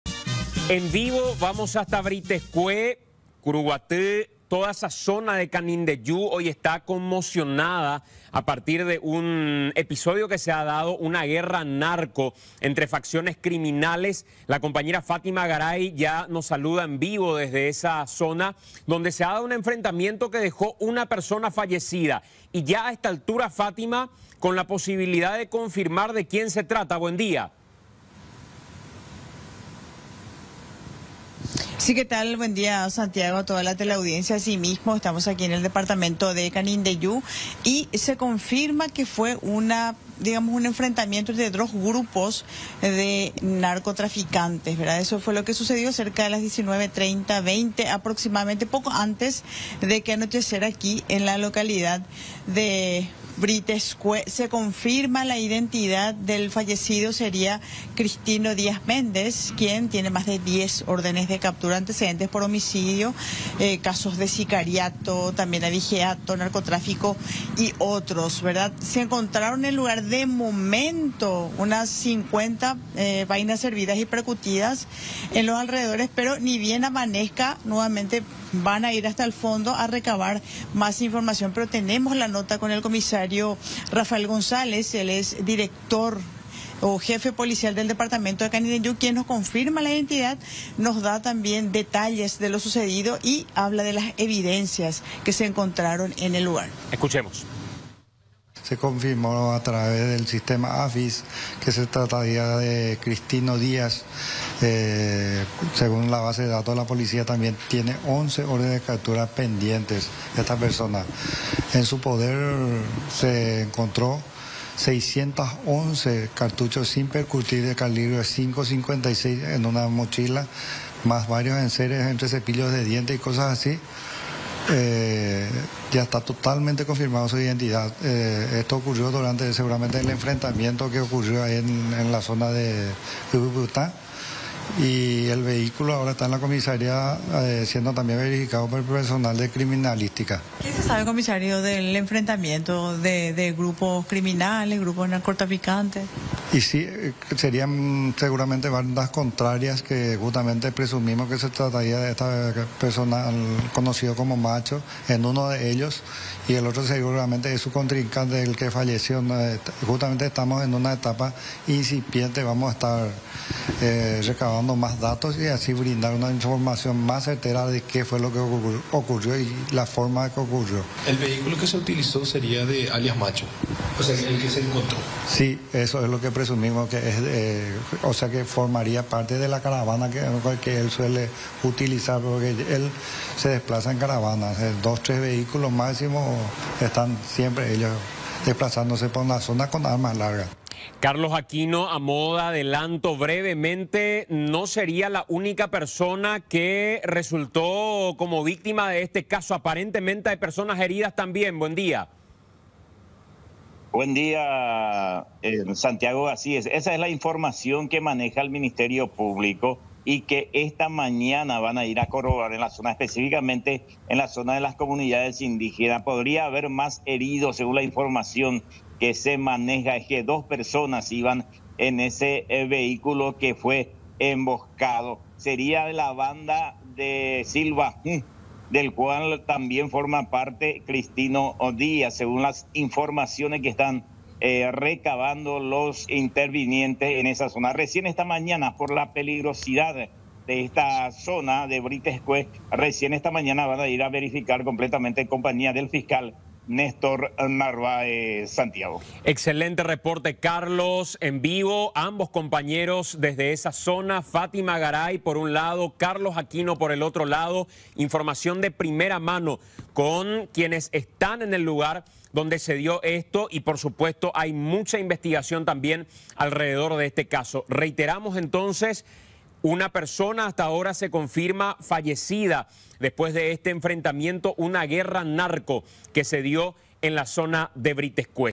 Guerra narco deja un muerto en Brítez Cué
Dos grupos narcos se enfrentaron y una persona falleció en la ciudad de Brítez Cué, departamento de Canindeyú. Se trata de Cristino Díaz Méndez, con 11 órdenes de captura pendiente y frondosos antecedentes por distintos hechos.